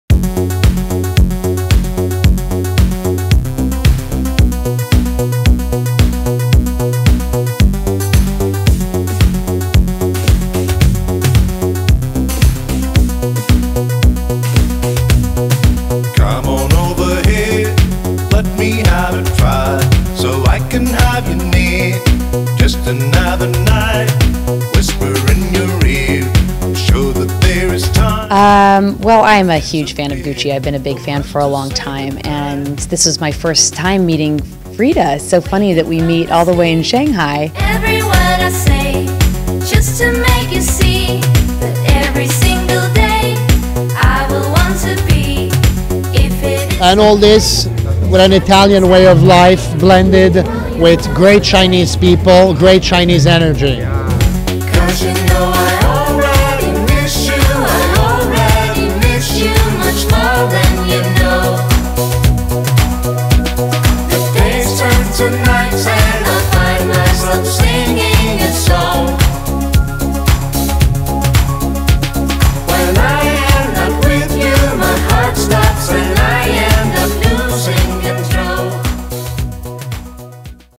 One Night in Shanghai: Gucci Club
For one night only in Shanghai, Gucci opened the doors to the impossibly chic Gucci Club.